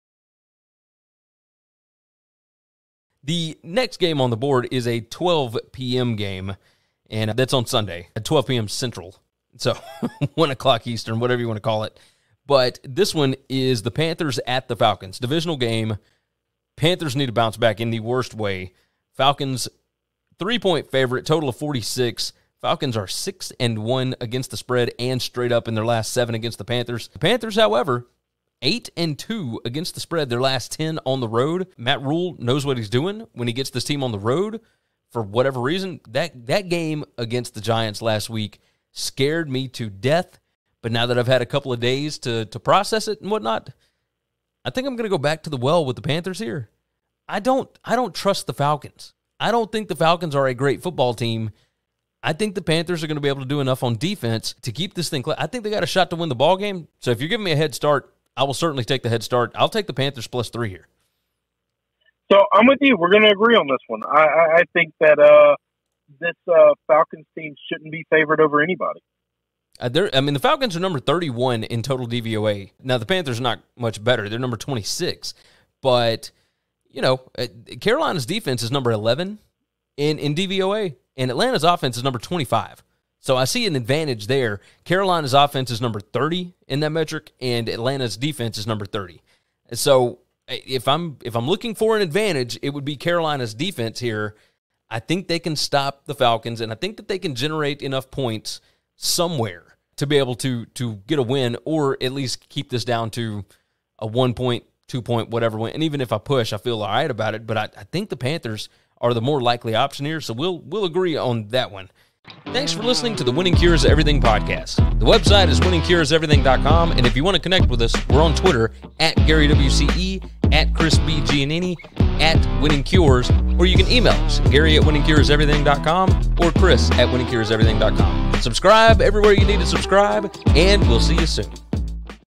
Carolina Panthers vs Atlanta Falcons 2021 NFL Week 8 Picks Against the Spread
Gary & Chris give a quick Carolina Panthers vs Atlanta Falcons preview, predictions & their picks against the spread (ATS) for the NFL Week 8 matchup this weekend!